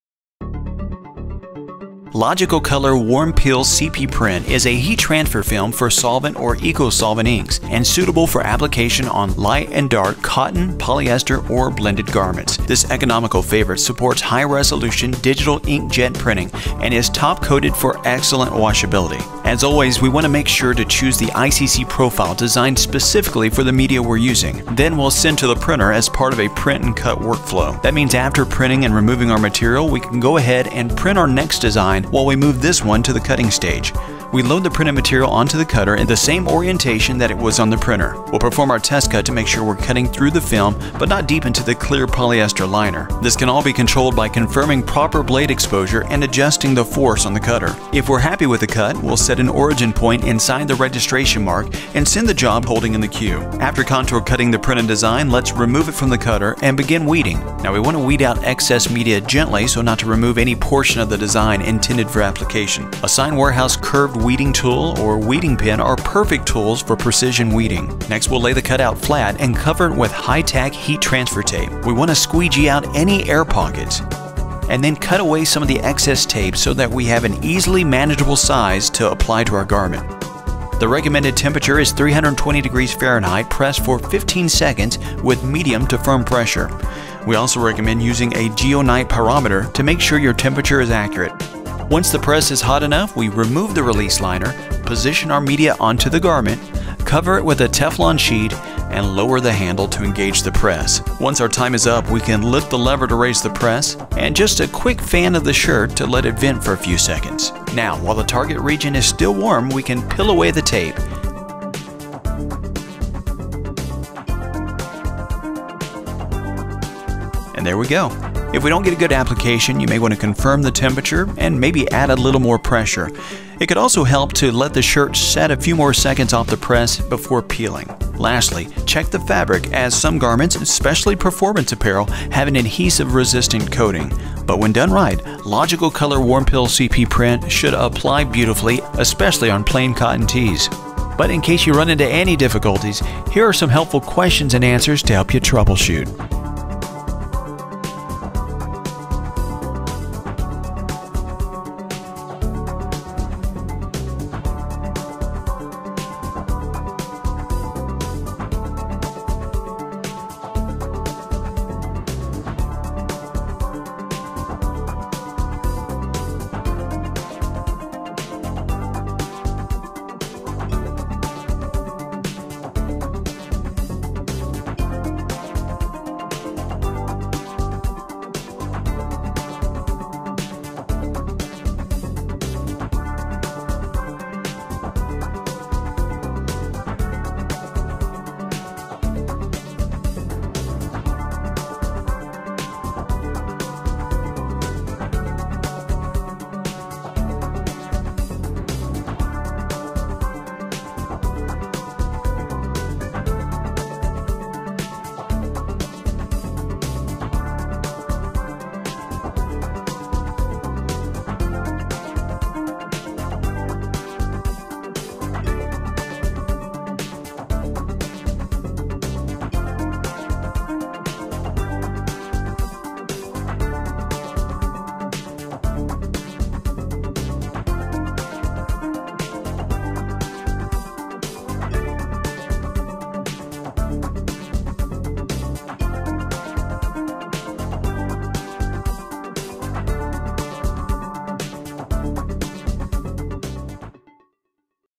How to Print and Cut Logical Color WarmPEEL CP PRINT Solvent Printable T-shirt Vinyl
This video will teach you how to use your new Logical Color WarmPEEL CP Print T-shirt vinyl, both with printing and cutting. It is perfect for application on light or dark garments. Logical Color WarmPEEL CP Print is very easy to cut and weed, making creating the perfect shirt an easy task!